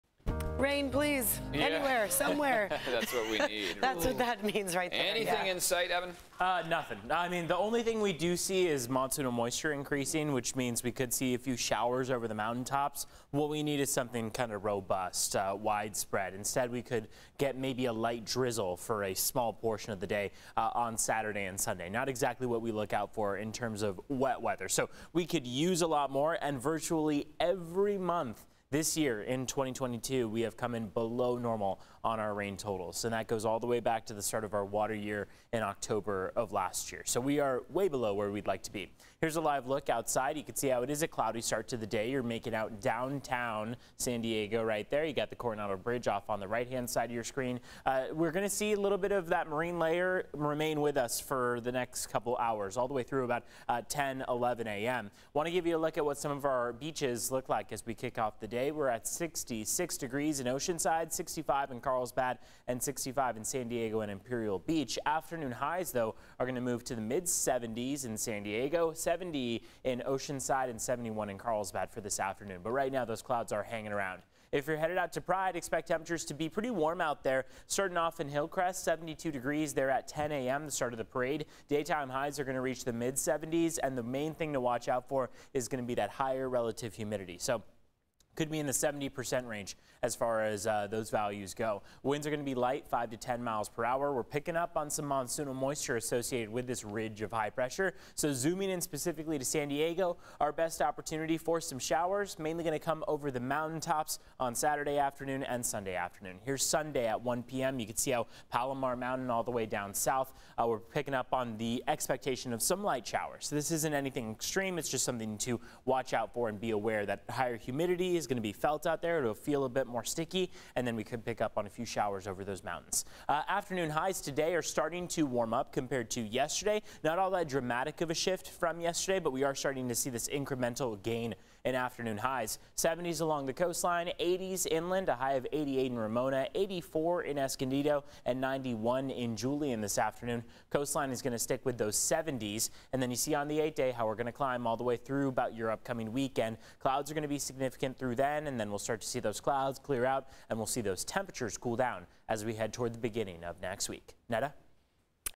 Warmer, humid weather continues through the weekend
As temperatures warm up, humidity will also rise, leading to a slight chance of mountain and desert thunderstorms this weekend.